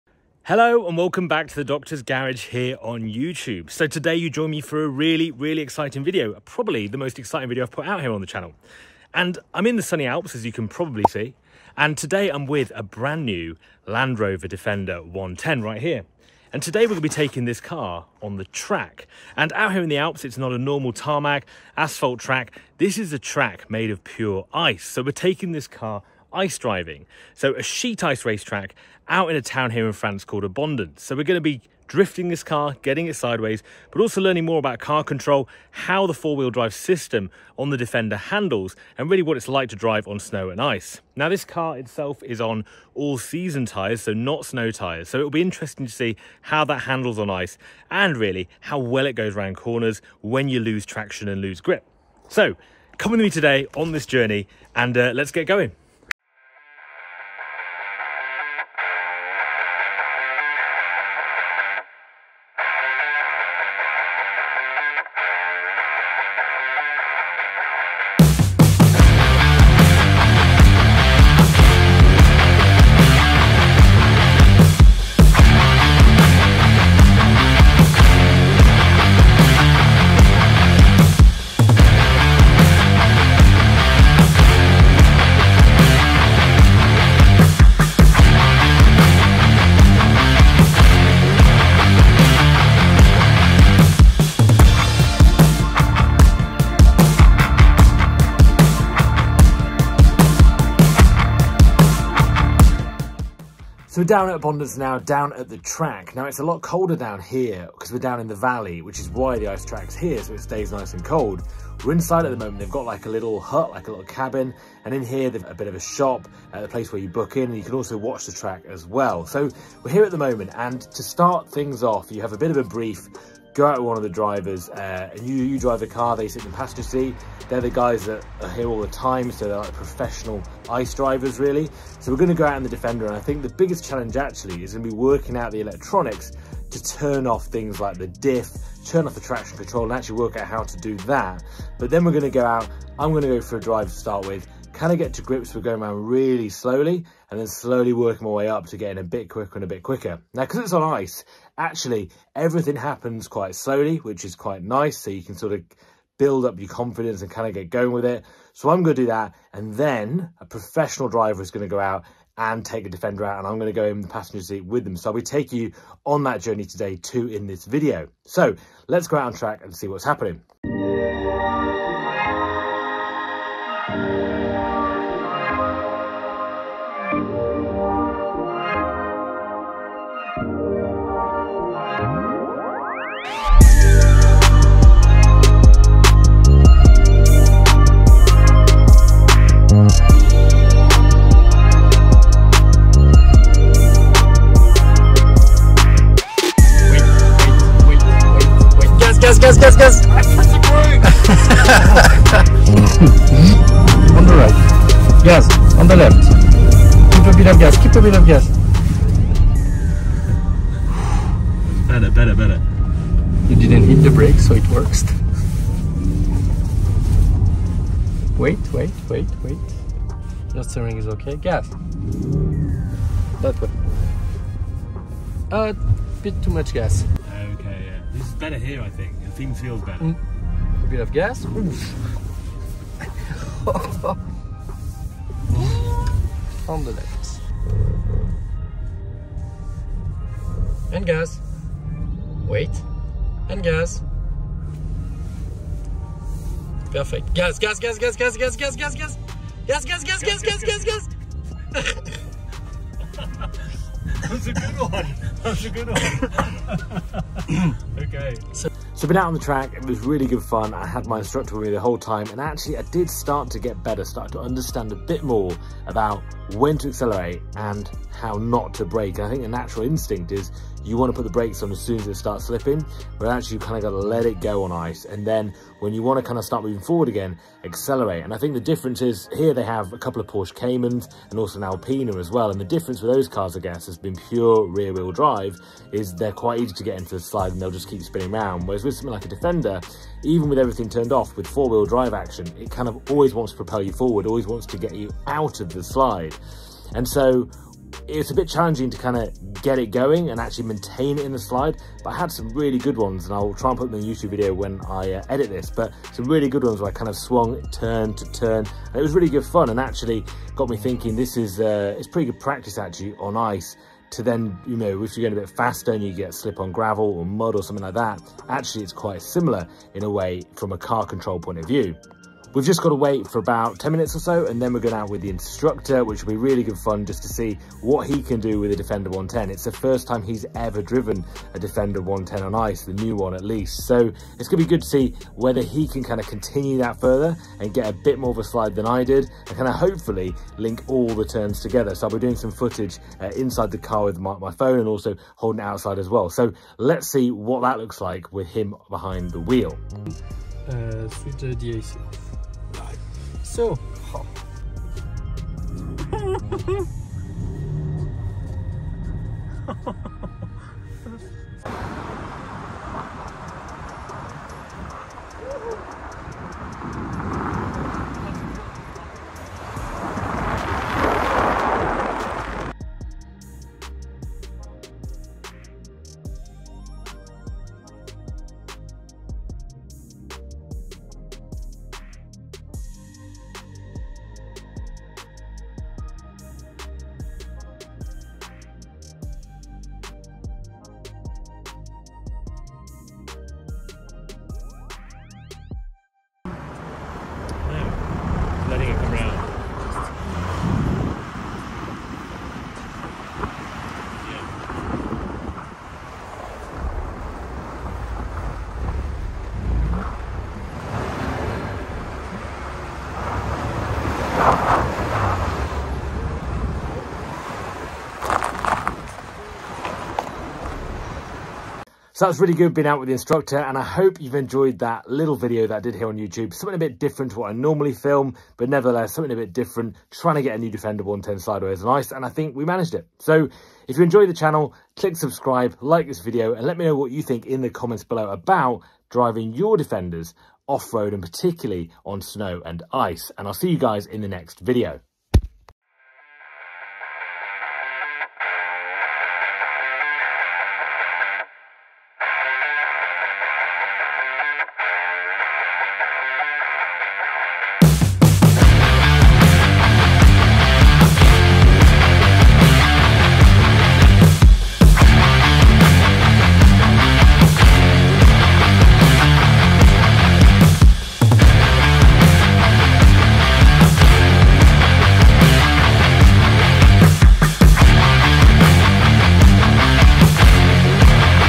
NEW LAND ROVER DEFENDER 110 OFF-ROAD ON ICE!
In this video I take the brand new Land Rover defender p400e on ice! We visit the circuit at Abondance in the French Alps and learn how to drift the new defender and about how good the new Land Rover defender is off road on snow and ice.

The new Land Rover defender in this video is the hybrid model the p400e with 27 miles of electric range.